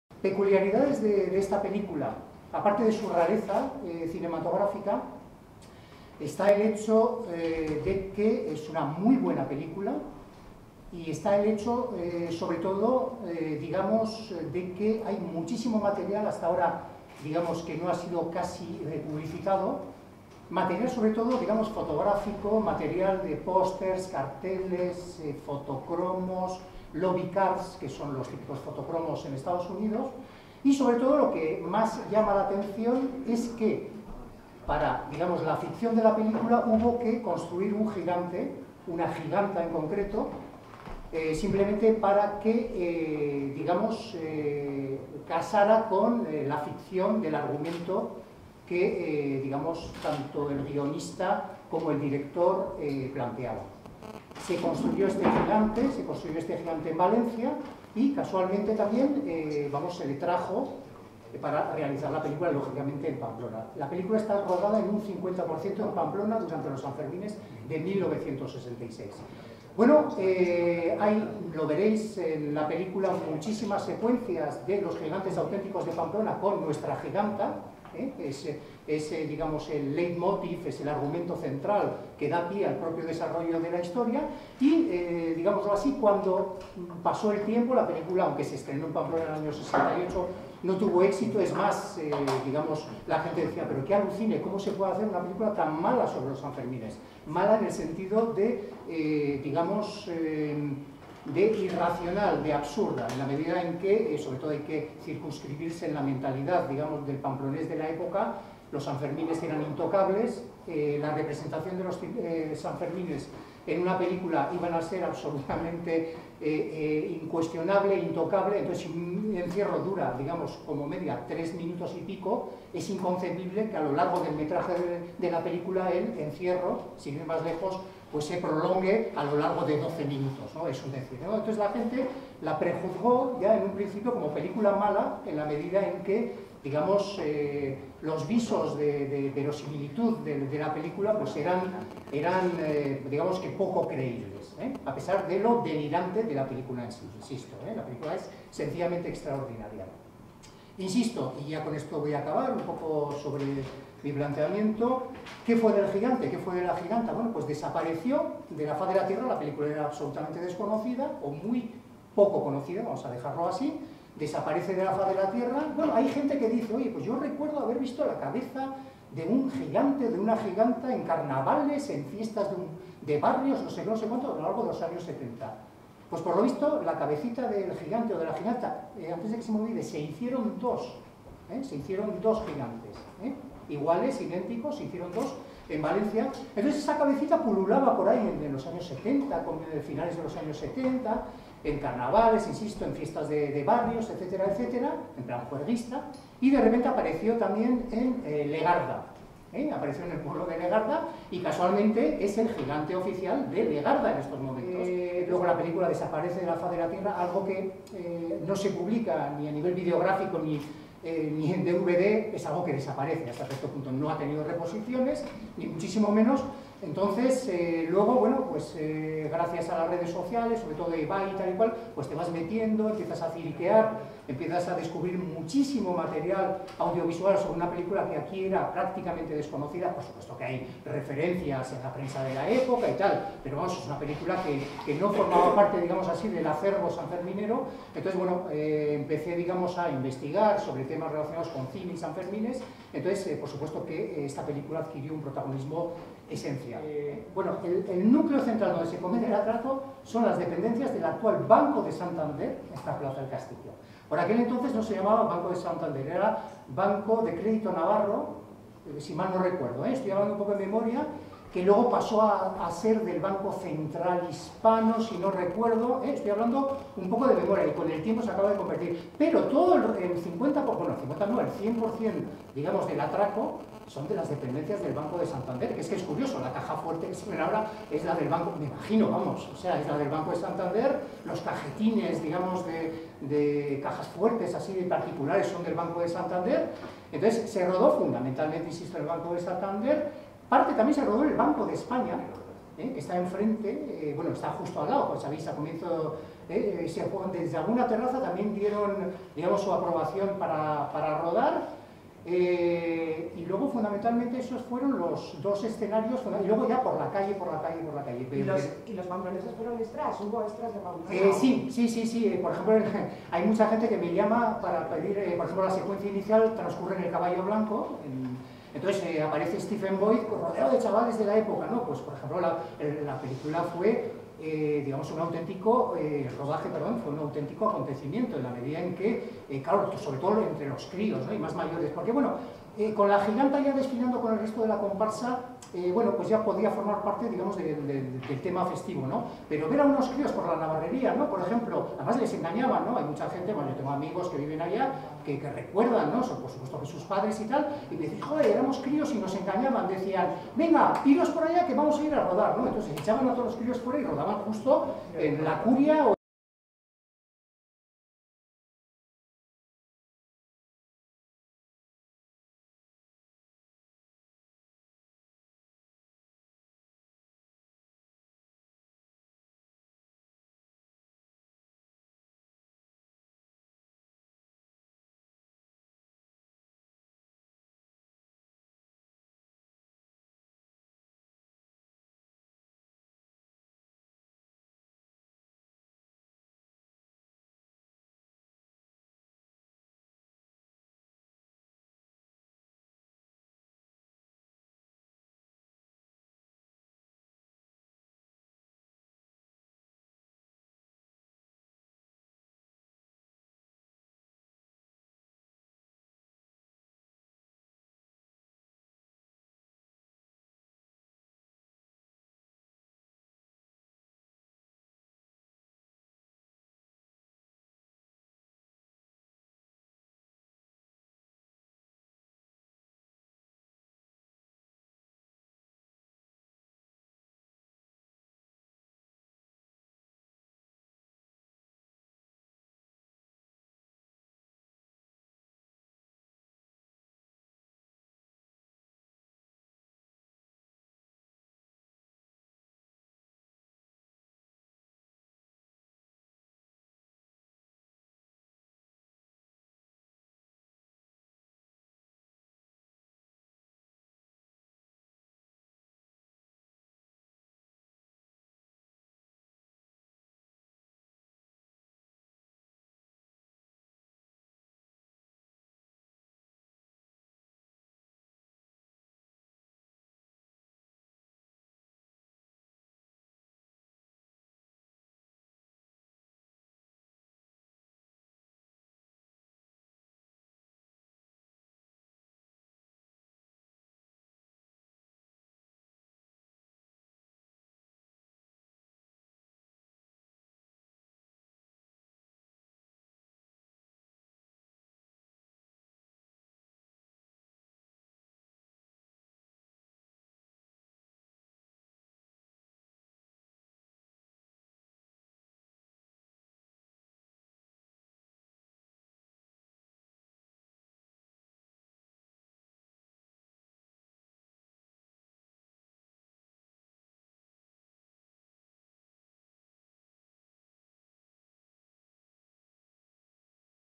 Exposición de la película "Carnaval de Ladrones" en la "Semana cultural de las peñas"
Exposición de la película rodada en Sanfermin "Carnaval de Ladrones" en la  "Semana cultural de las peñas"